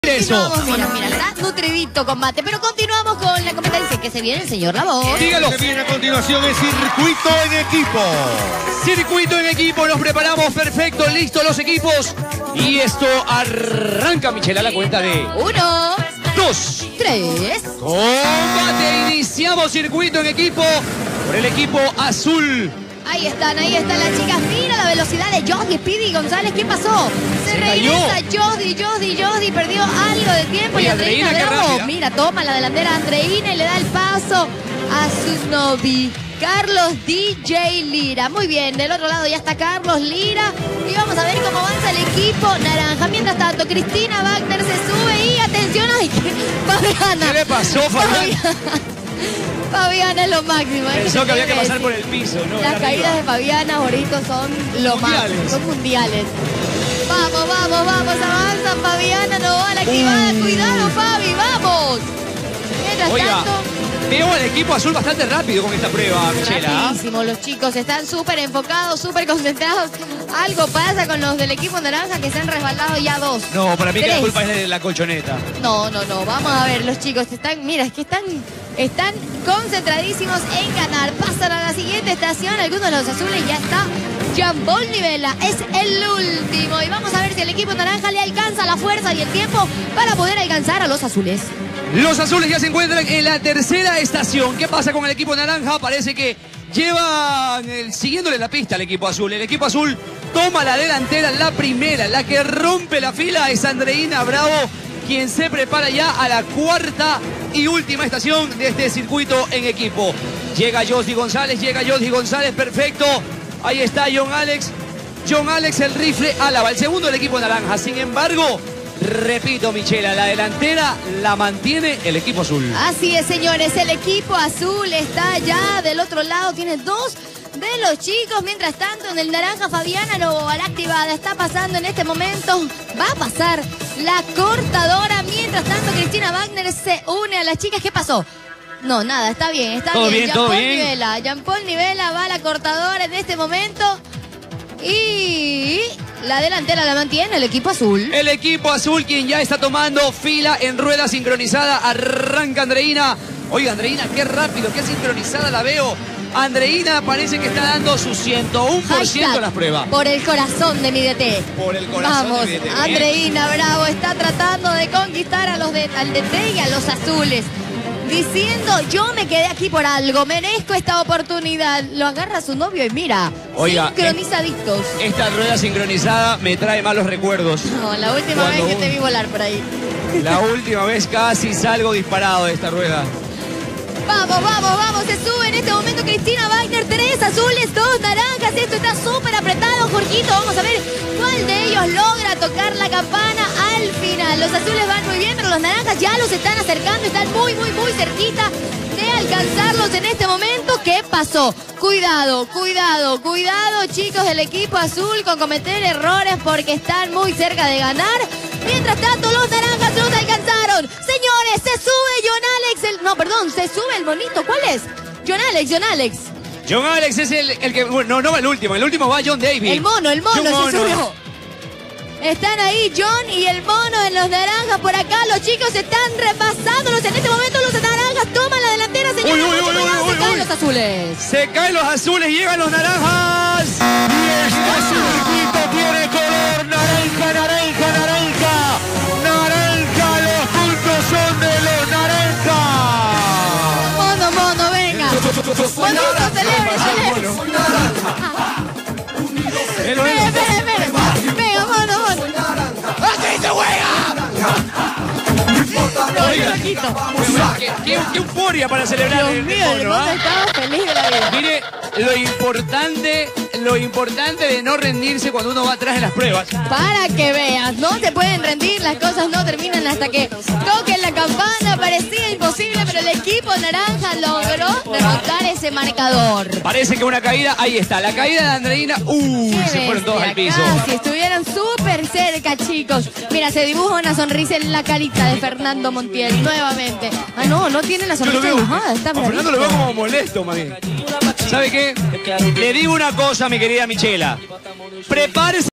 Los combatientes se enfrentaron en Circuito en Equipo
Los puntos fueron para los naranjas. Los azules estuvieron muy cerca durante la competencia pero los naranjas sacaron su fuerza y se llevaron los puntos de la prueba.